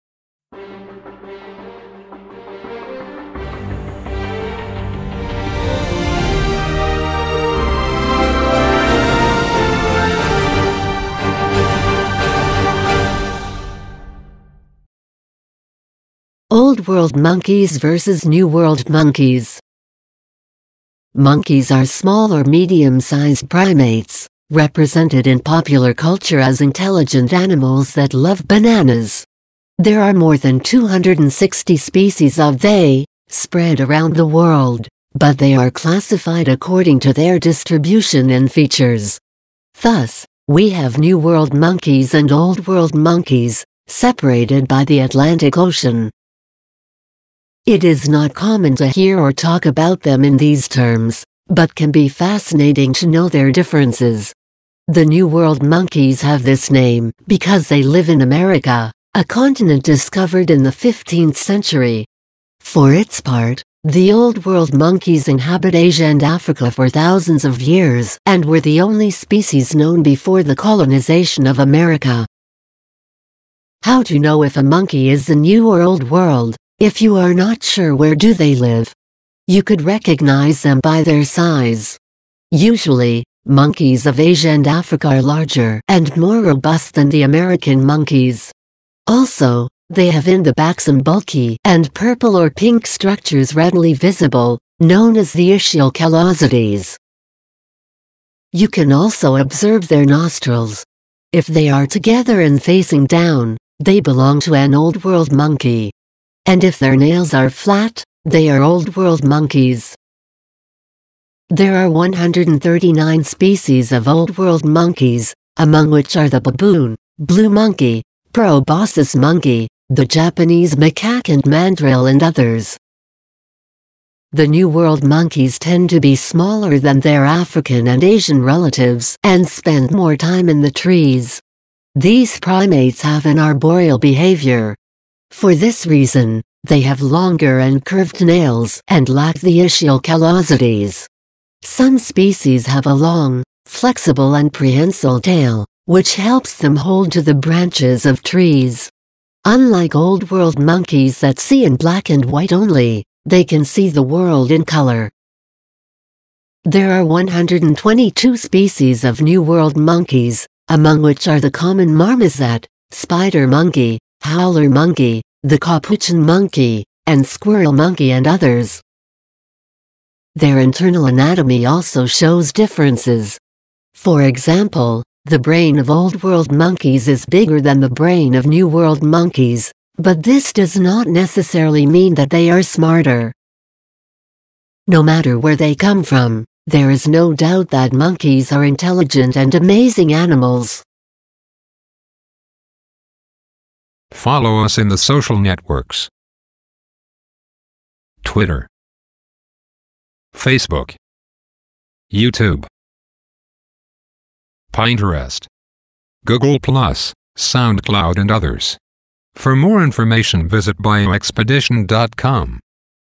Old World Monkeys Vs. New World Monkeys
Similarities and differences between Old World Monkeys and New World Monkeys.